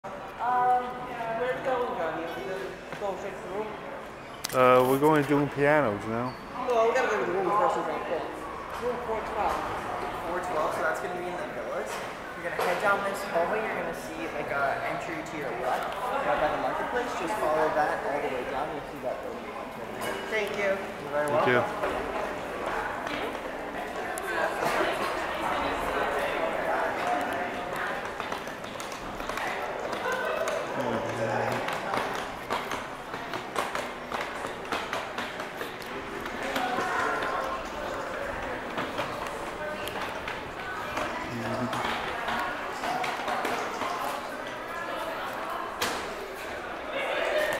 Entering Disney's Beach club resort
We were staying in the Boardwalk but we had one night at Beach club
Date: Thursday May 2nd 2024